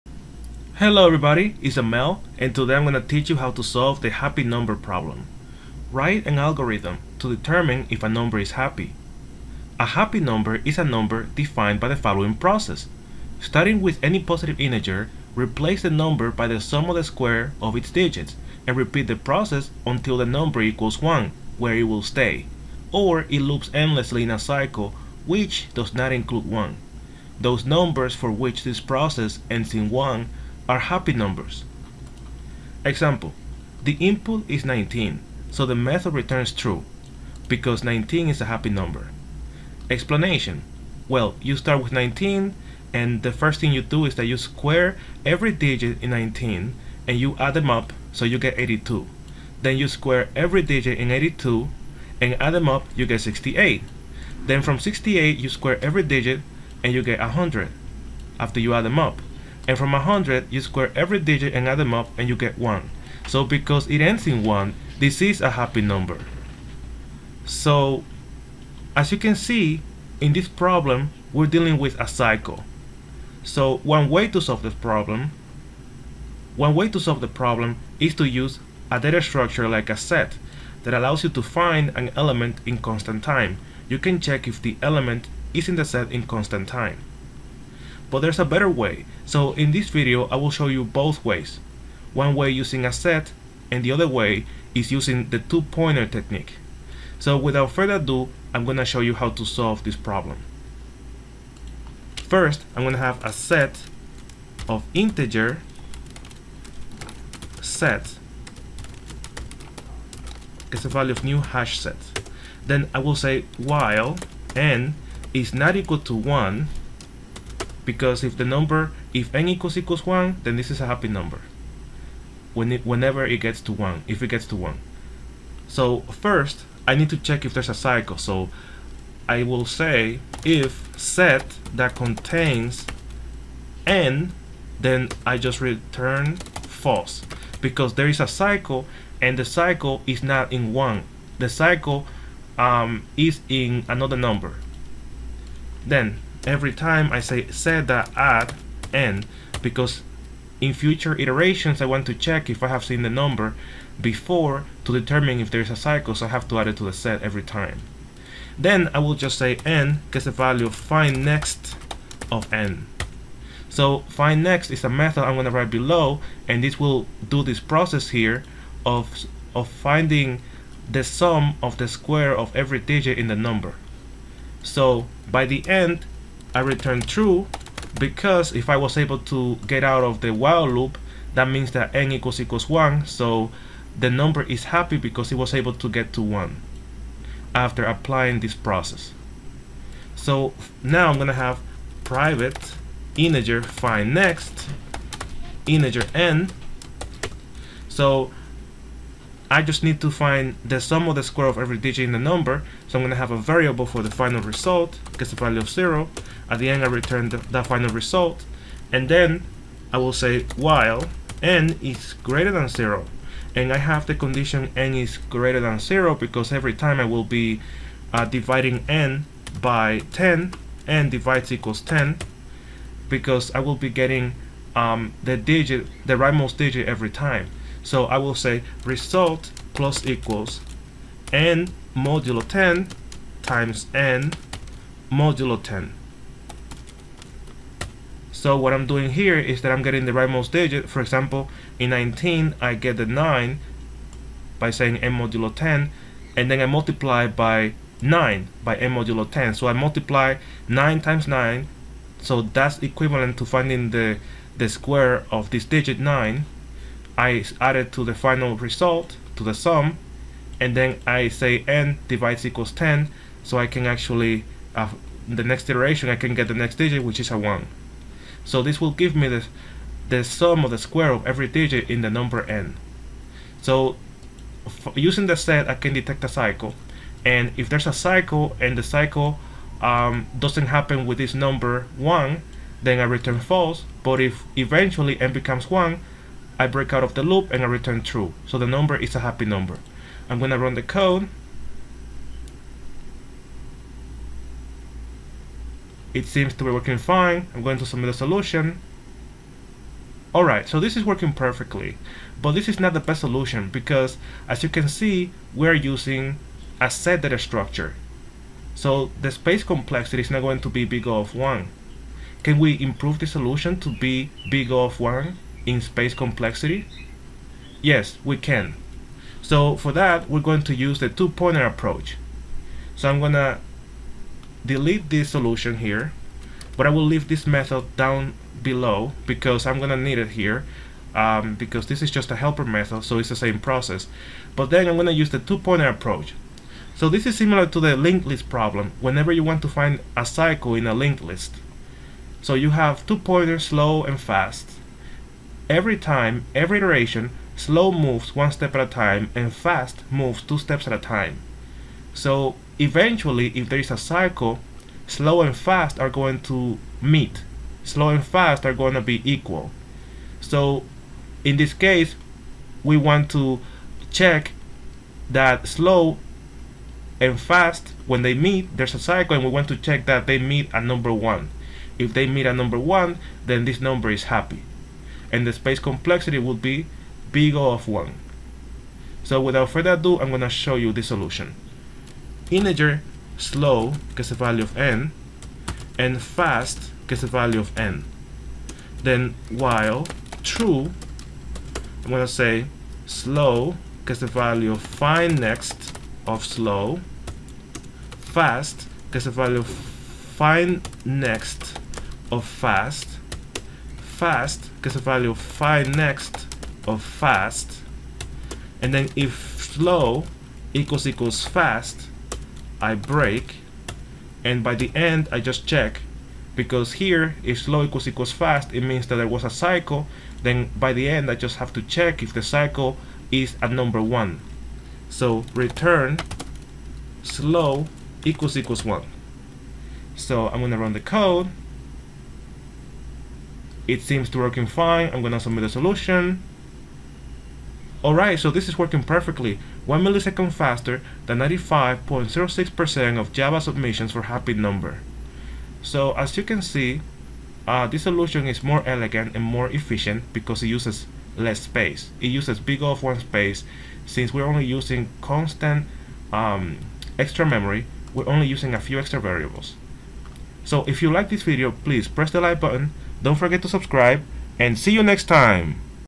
Coding Interview Tutorial 97 - Happy Number [LeetCode]
Learn how to check if a number is happy easily and efficiently!

Algorithms, data structures, and coding interviews simplified!
Ace the coding interview!

This is an important programming interview question, and we use the LeetCode platform to solve it.